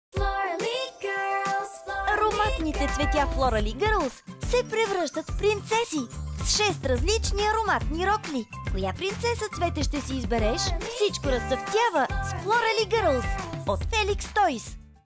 FLORALLY GIRLS Кукла-цвете 113460/113461
Ароматните цветя Florally Girls се превръщат в принцеси! С шест различни ароматни рокли! Коя принцеса-цвете ще си избереш? Всичко разцъфтява с Floraly Girls!